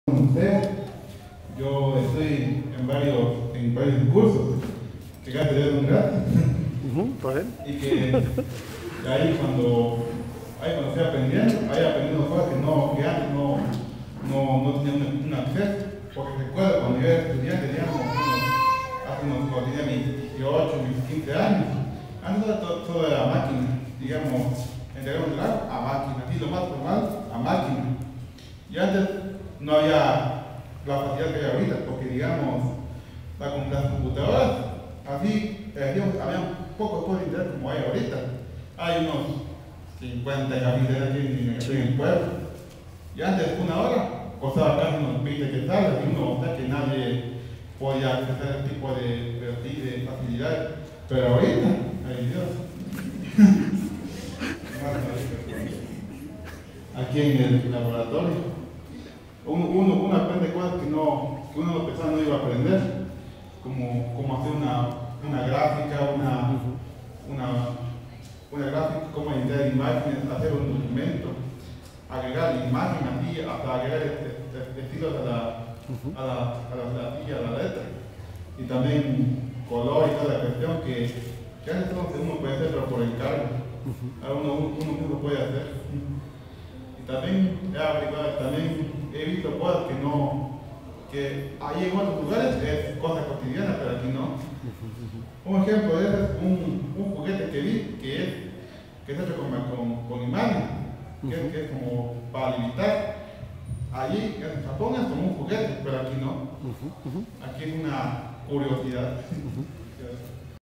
Clausura cursos CCD, palabras alumno
Patzún, acto de clausura de los cursos del Centro Comunitario Ditigal